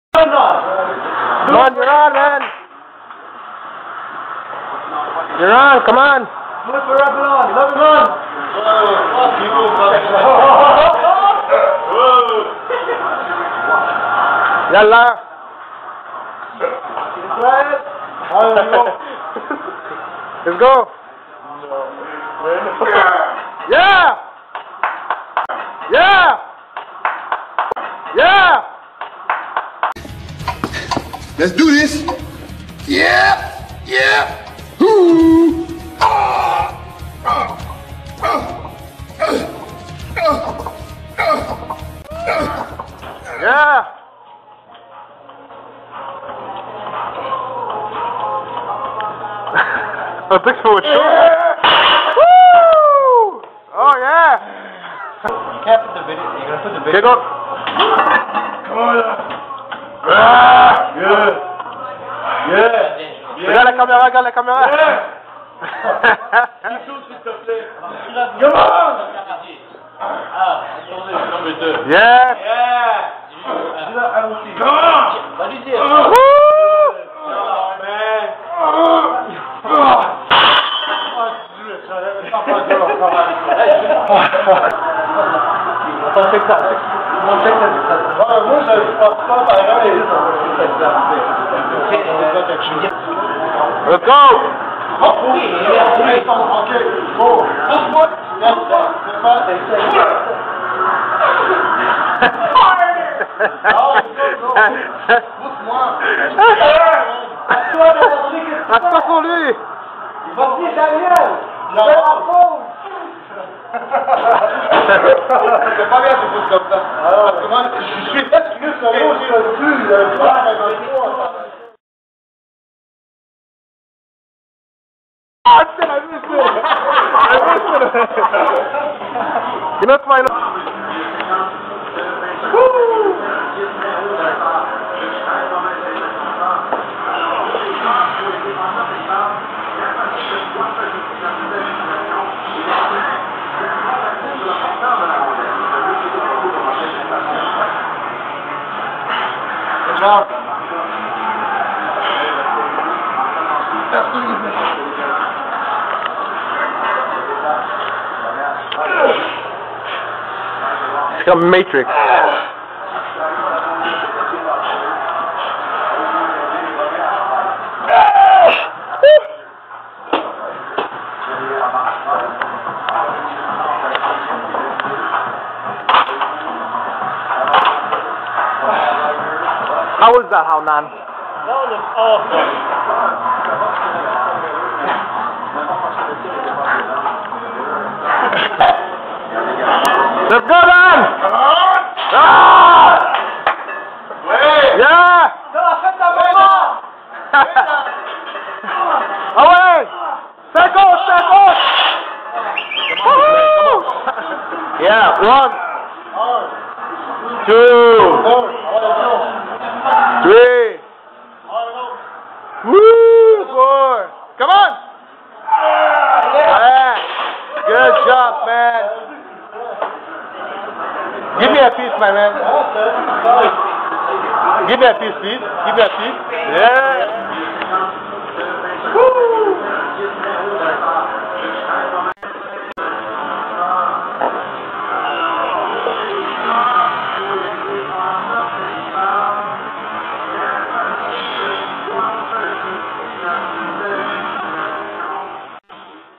Big strong men
me and my buds having fun at the gym